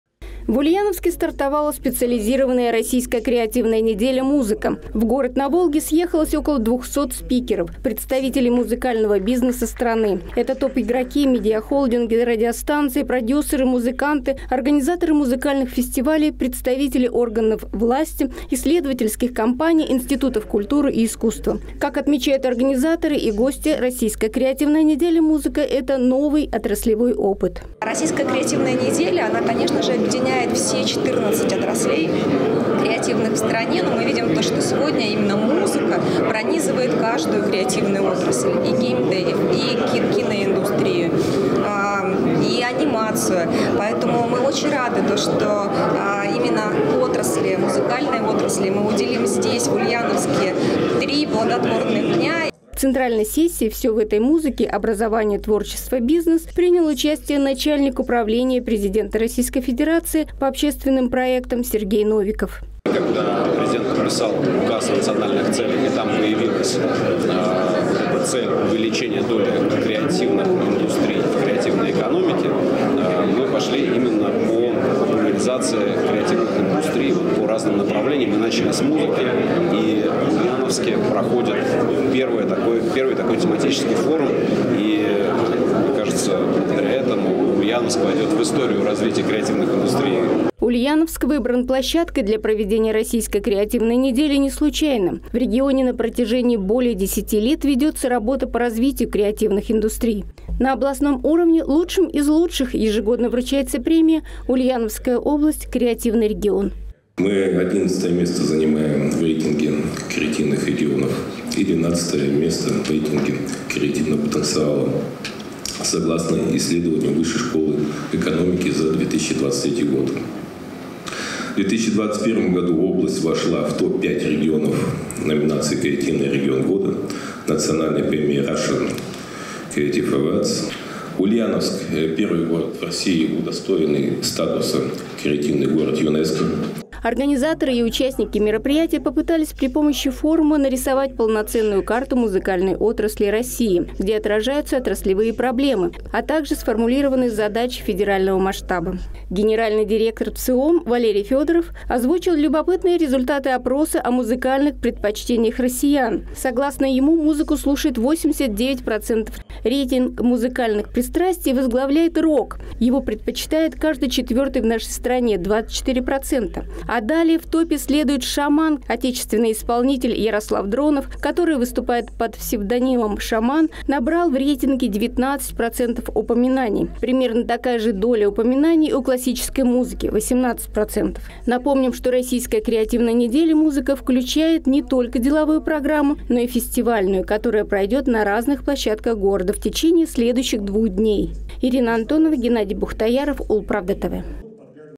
Рок и SHAMANизация. В Ульяновске проходит первая «Российская креативная неделя - Музыка»
В Ульяновске проходит главное федеральное событие в сфере креативных индустрий. Оно полностью посвящено музыке, поскольку именно эта креативная отрасль, как считают организаторы, влияет наибольшим образом на общество.